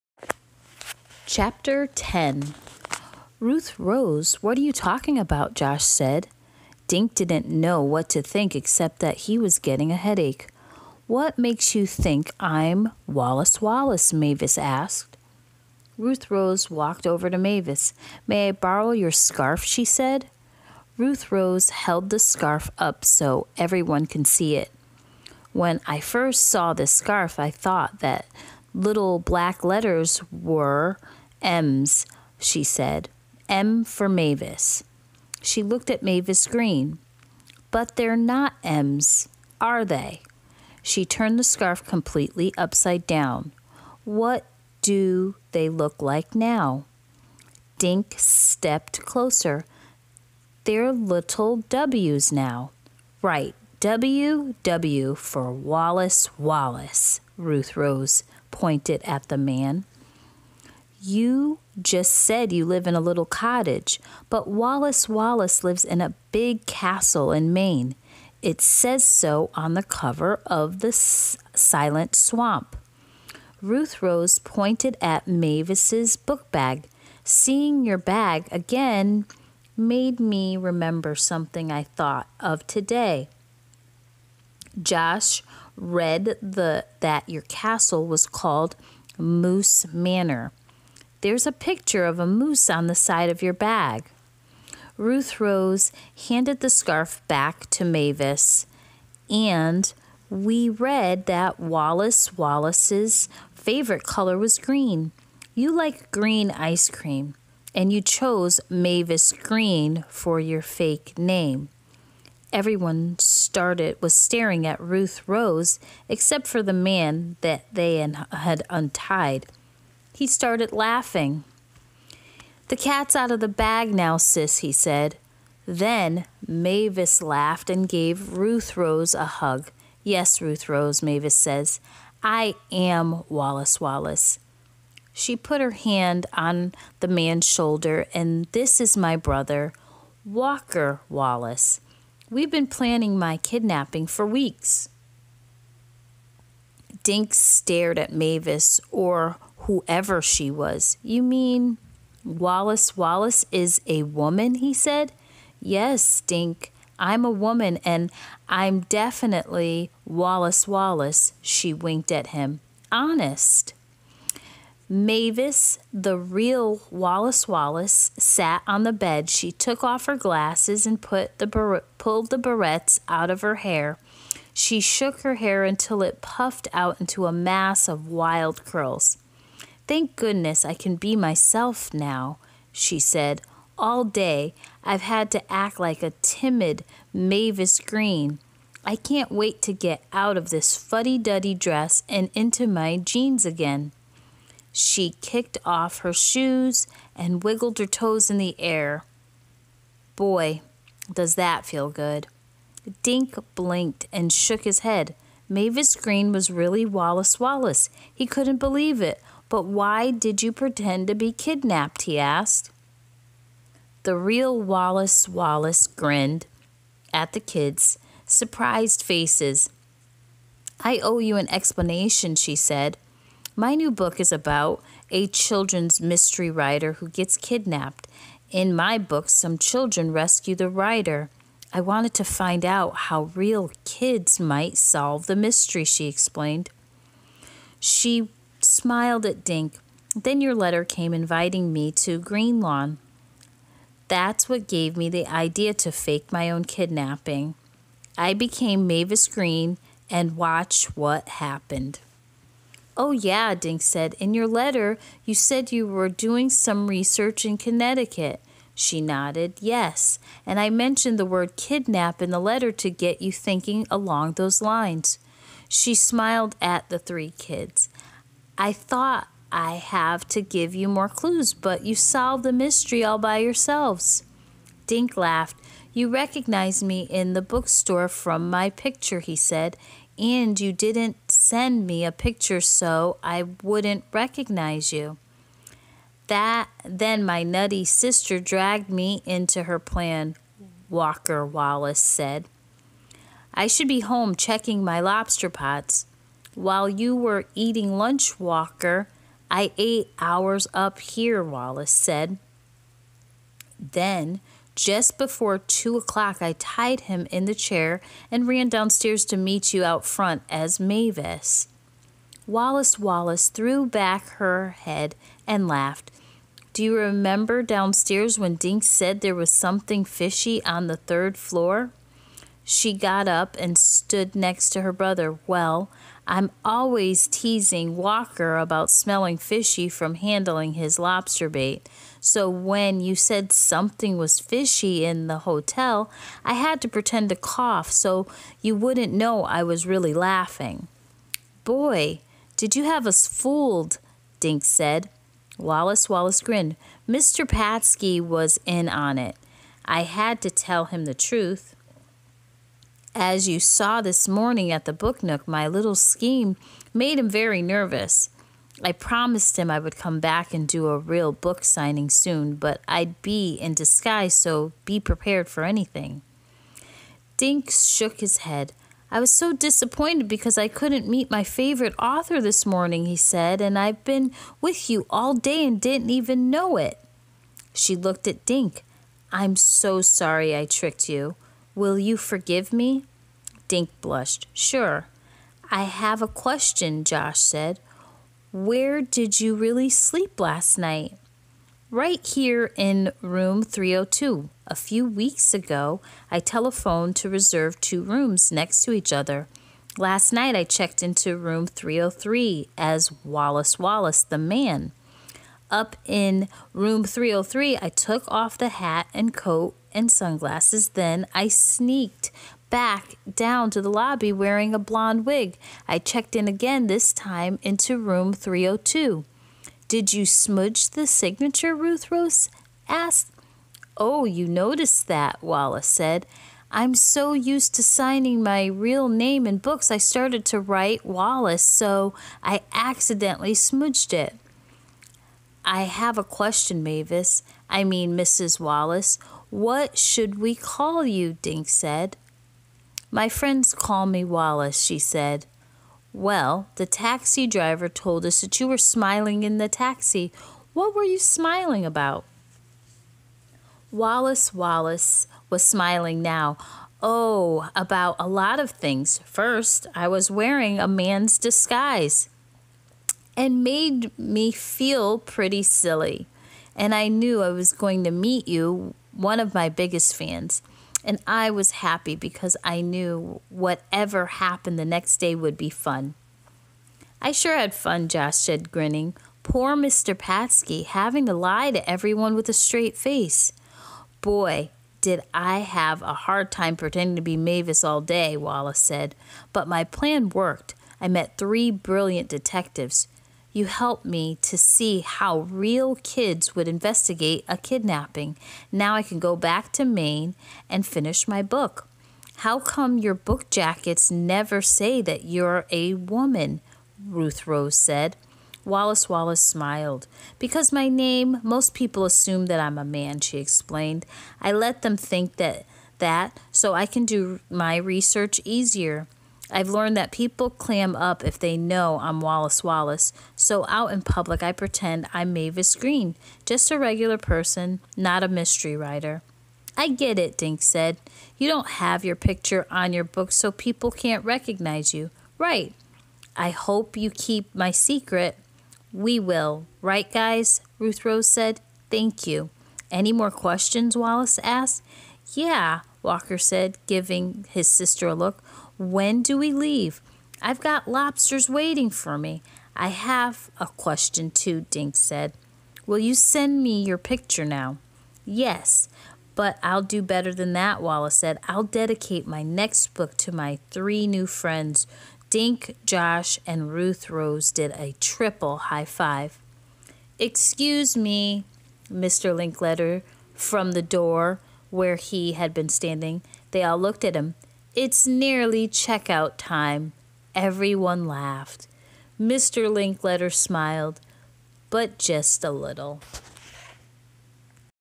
Chapter 10 A to Z Mysteries :The Absent Author
A is for Author . . . A famous writer is coming to Green Lawn! Dink rushes to the bookstore to meet his favorite author, Wallis Wallace, and get all his books signed. But the author never shows up! Where is Wallis Wallace? It’s up to Dink and his friends Josh and Ruth Rose to track him down.